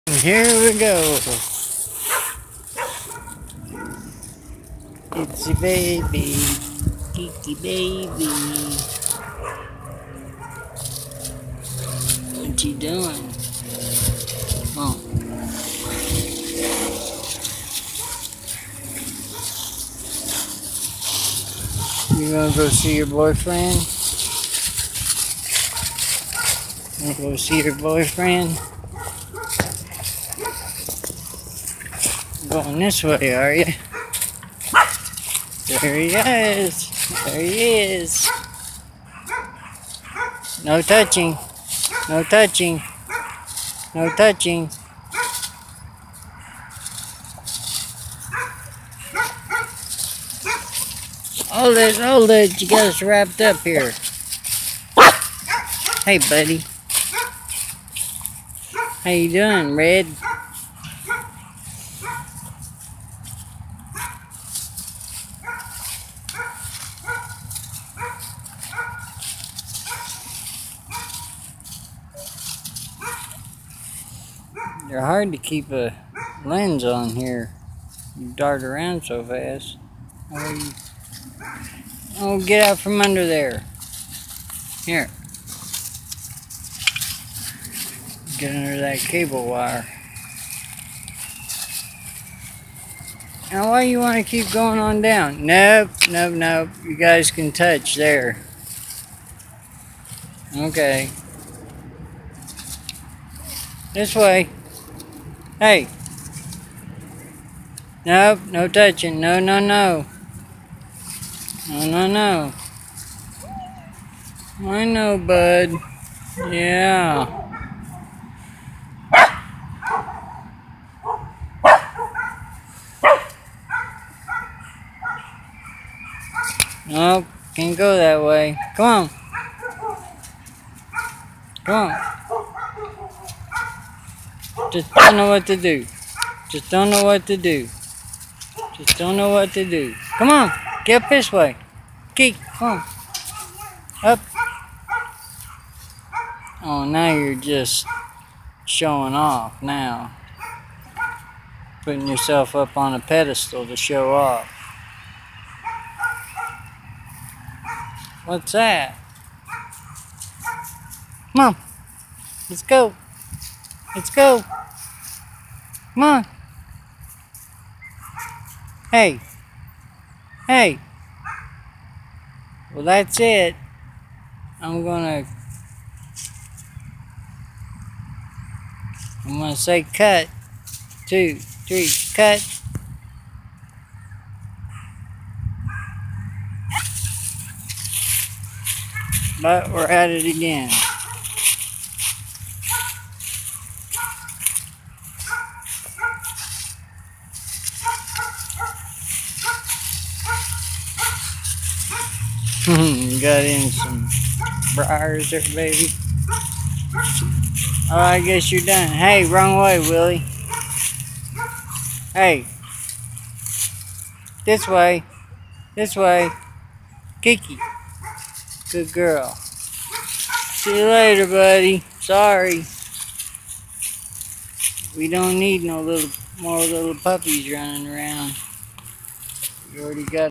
Kiki
Kimi showing off again in front of in front of her boyfriend. Again for the third day in a row.